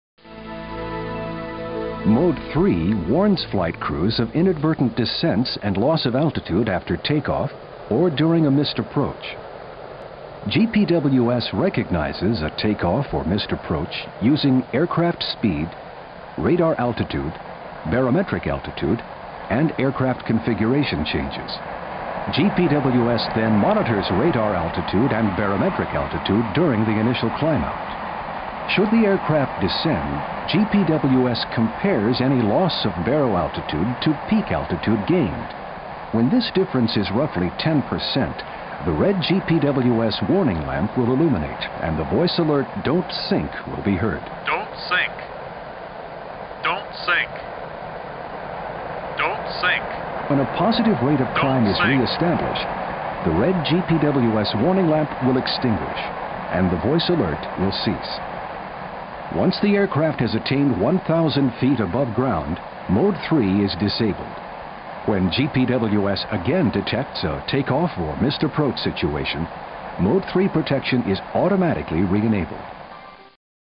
GPWS Mode 3 - Altitude loss after take off or with a high power setting
A training video using animation to explain the features of GPWS Mode 3 - Altitude loss after takeoff or with a high power setting ("DON'T SINK").

Check out more study material for flight crew your cockpit reference codex.